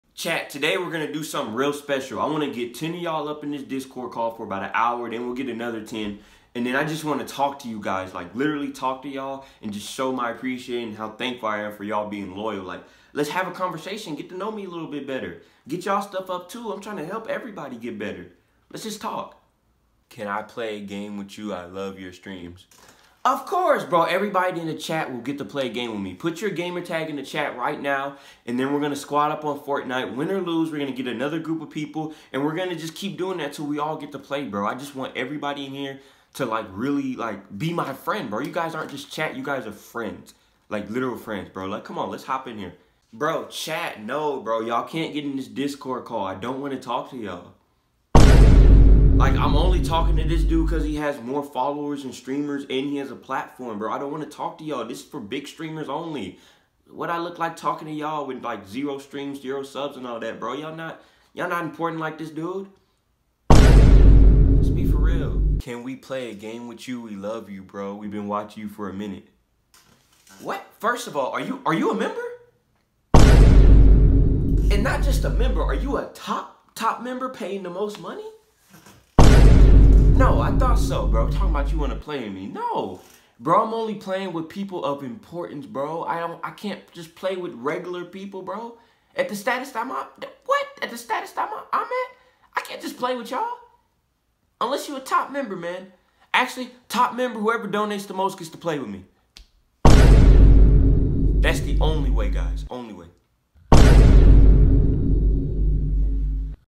Small Streamers Interacting With Chat Vs Big Streamers Interacting With Chat…
Big streamers only play if you pay Fr 😭😭